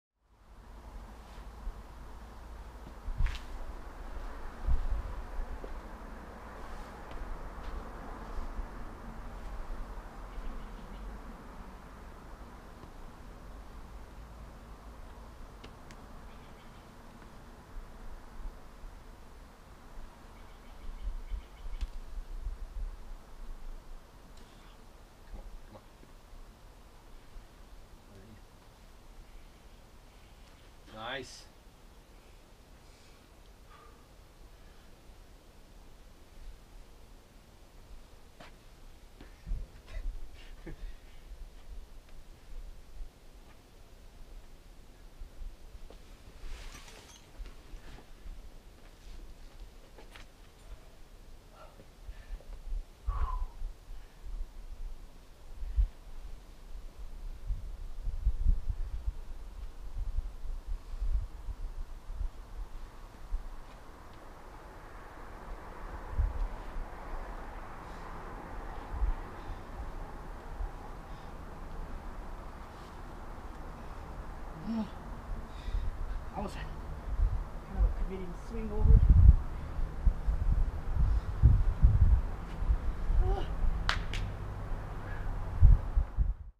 Tuolumne Meadows Bouldering - The Arete V4 - Carlos
Carlos powers his way through this fun roof problem.